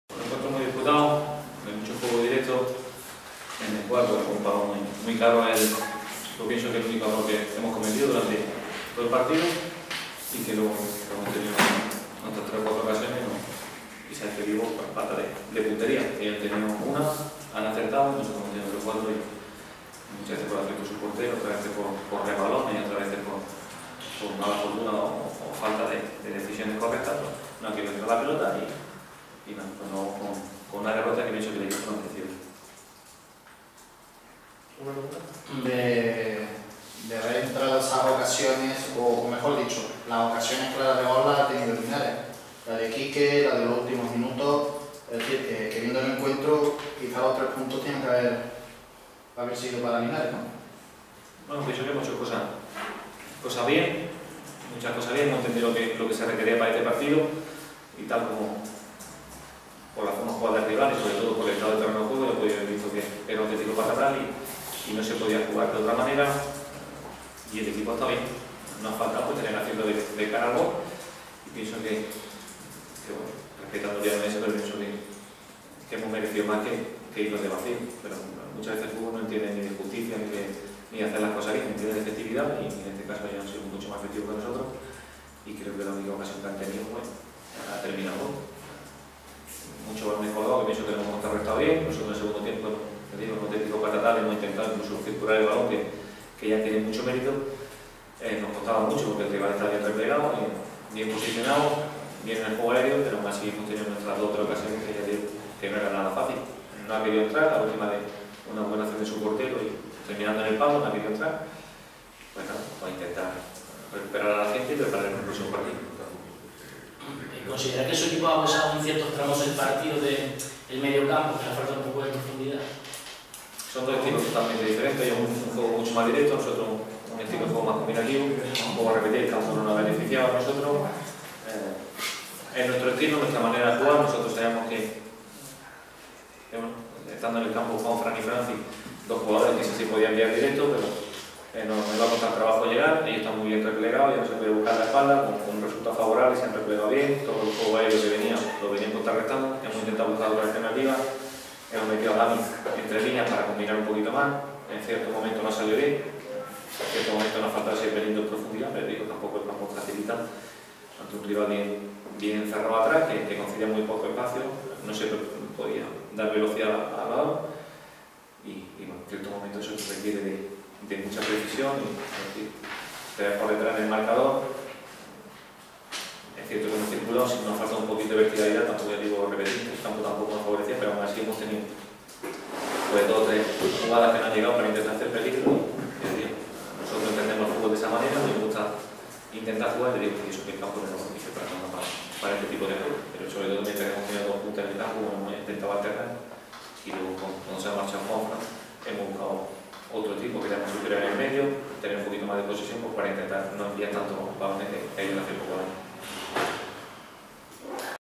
J5 RP Antonio José García
Rueda de Prensa de Antonio José García, entrenador del Linares Deportivo tras la derrota por 1-0 ante el C.F. Villanovense en la quinta jornada de Liga